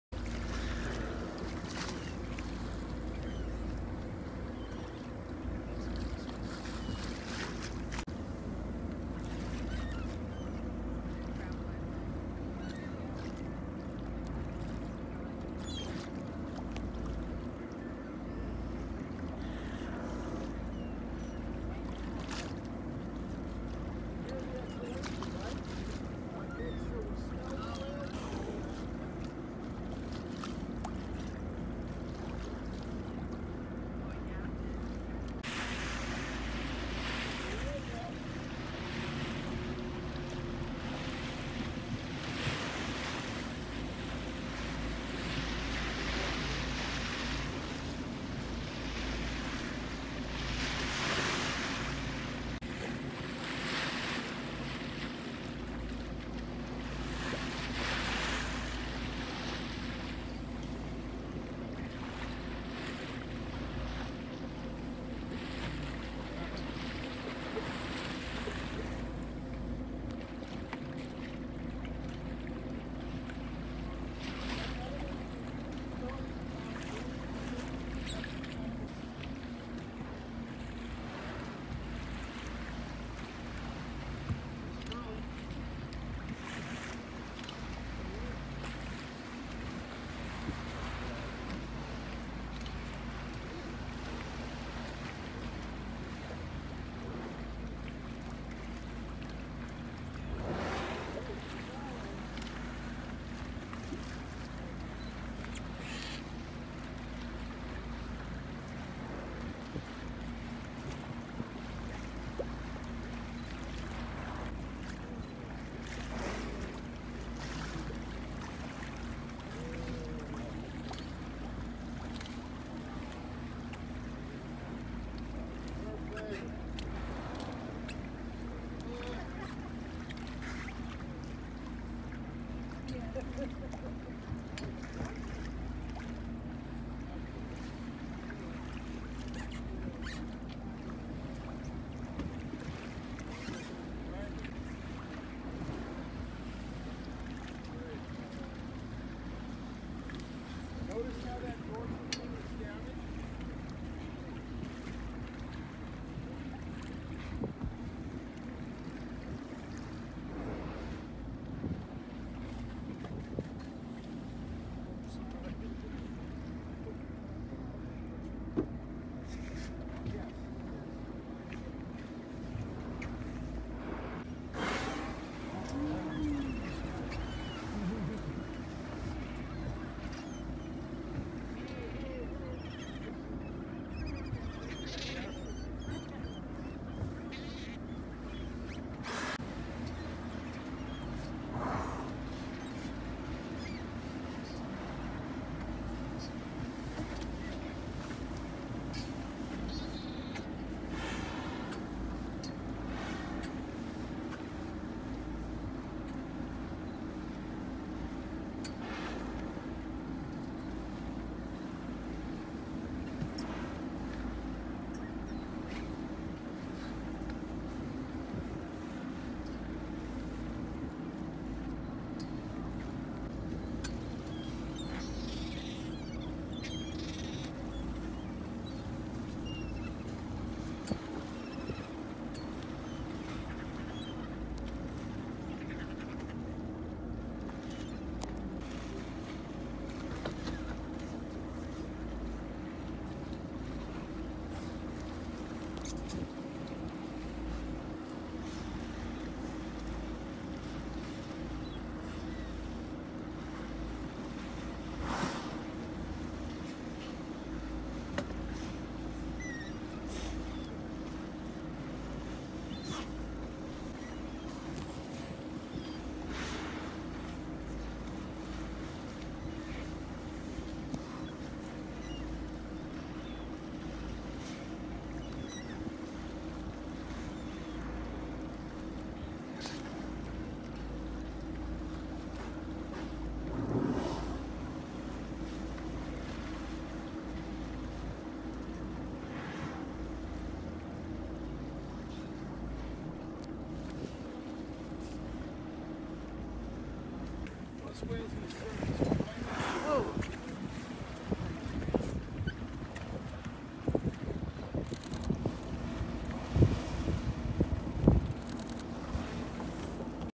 Humpback whales, seals and gulls (Farallon Islands, San Francisco, California)
Humpback whales, California seals and gulls feasting off the Farallon Islands (Farallon Marine Wildlife Refuge), off the coast of San Francisco in the Pacific Ocean.